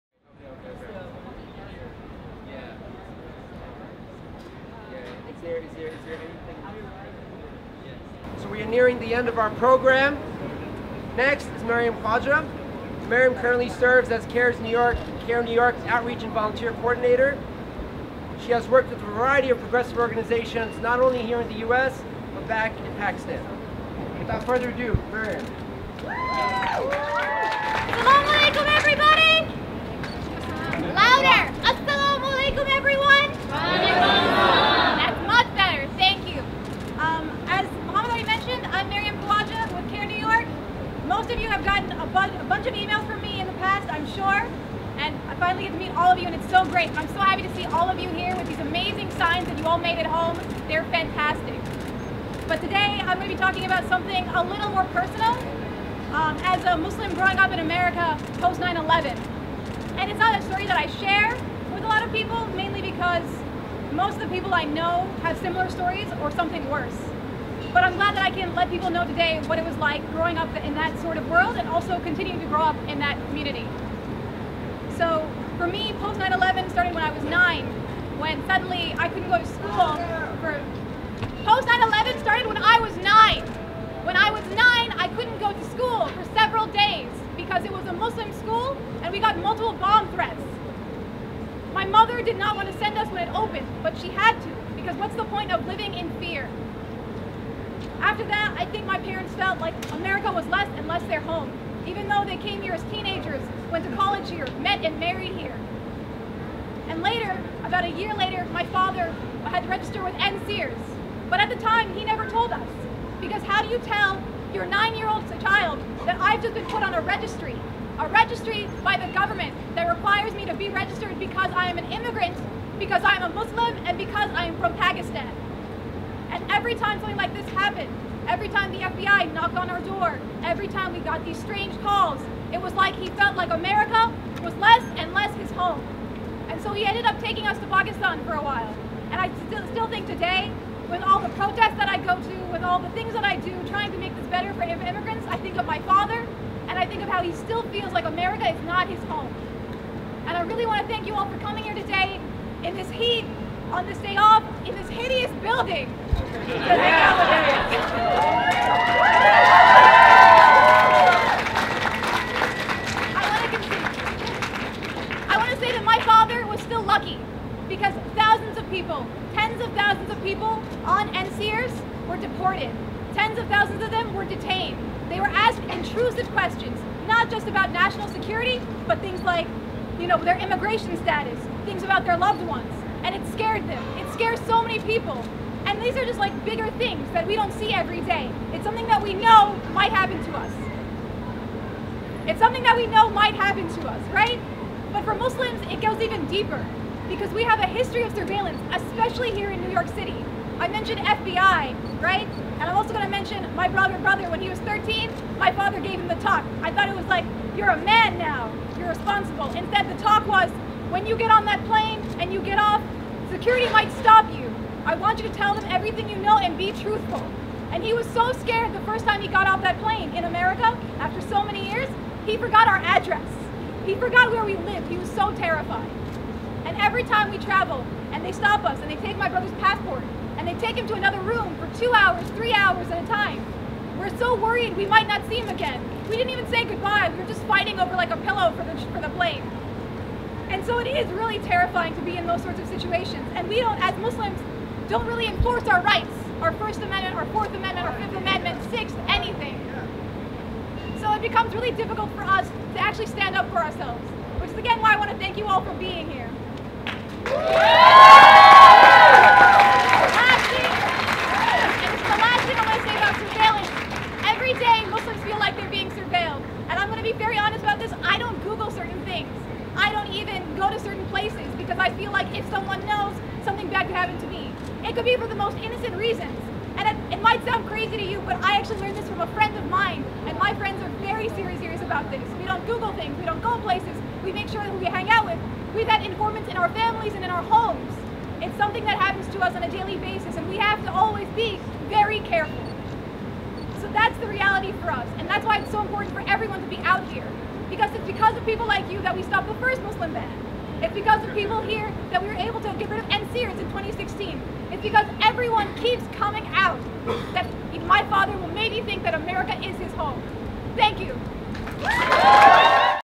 "We Will Not Be Banned" Presser" - Marium Khawaja CAIR NY
"We Will Not Be Banned: The broken promise of equal justice" - Speaker, Marium Khawaja (Pakistan) Outreach and Volunteer Coordinator, CAIR NY. JDL counter protest continued through out the presser.

On July 4th, 2017 (Noon) Trump Tower. They gathered on the fifth floor - a public space in the now infamous Trump Tower. Their purpose was to highlight how (they claim) the United States has repeatedly broken her founding promises and (they claim) targeted groups on the basis of race, religion, or ideology.

Speakers from a diverse array of organizations insist their communities have historically been targeted - from the U.S.’s internment of Americans of Japanese descent, to U.S.’s treatment of Puerto Rico, to President Trump’s Muslim Ban.